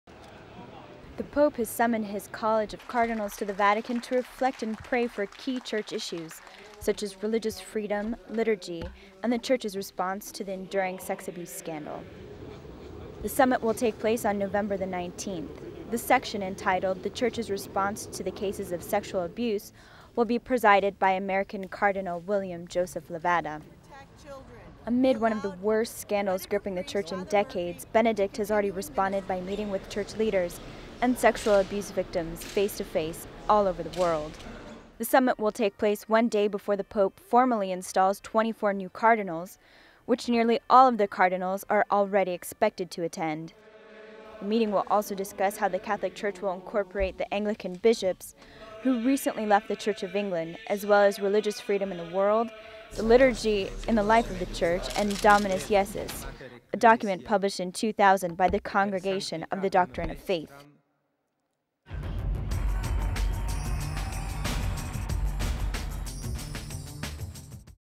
Pope calls cardinals worldwide to Rome to discuss response to sex abuse cases
The Pope has summoned his college of cardinals to the Vatican to reflect and pray for key church issues, such as religious freedom, liturgy of the word and  the Church's response to the enduring sex abuse scandal.

The summit will take place on November 19th.  The section entitled "The Church's Response to the Cases of Sexual Abuse," will be presided by  American Cardinal William Joseph Levada. .